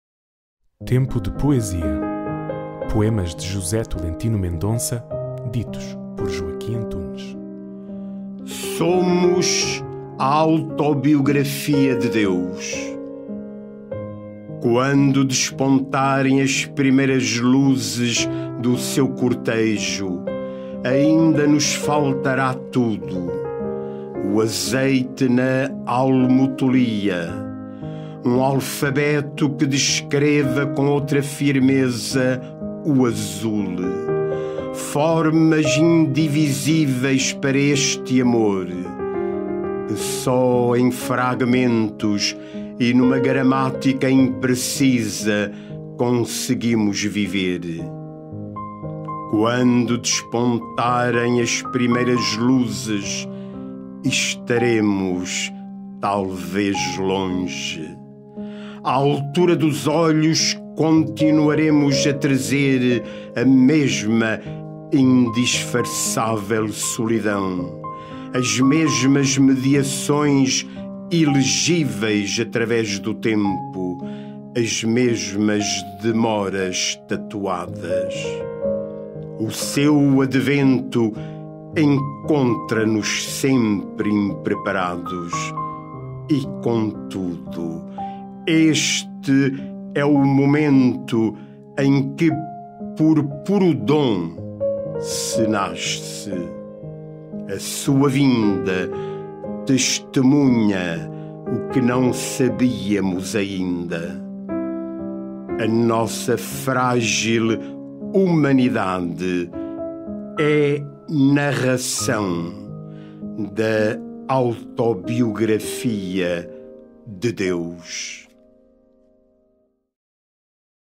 Autobiografia de Deus
Tempo de Poesia é um podcast do Boletim Salesiano, lançado semanalmente à quinta-feira na newsletter "Boletim Salesiano Hoje". Uma seleção de poemas de José Tolentino Mendonça, ditos por Joaquim Antunes.